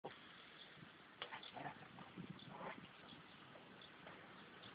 Portuguese Weimaraners
Lester and Idunas playing